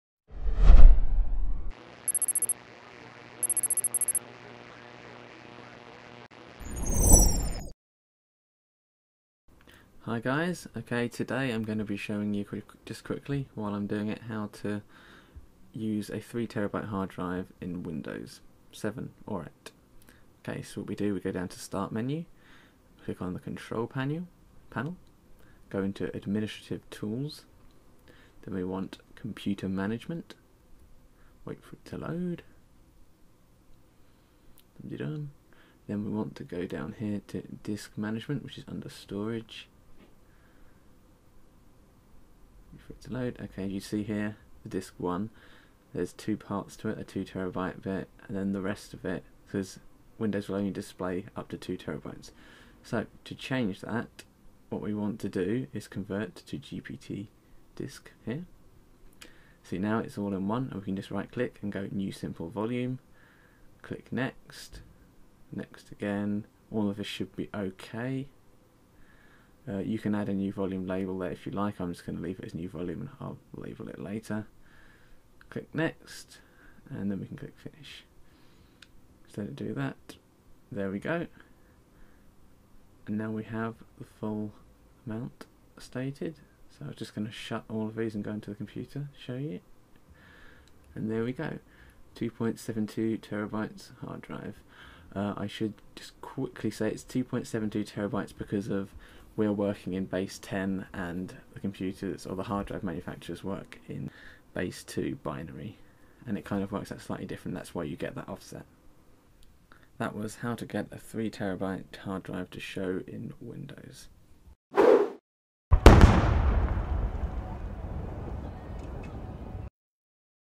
How to use a 3TB HDD with Windows
Just  a quick how to video while I was activating a 3TB HDD for my system. This will of course work with most other size HDDs over 2TBs.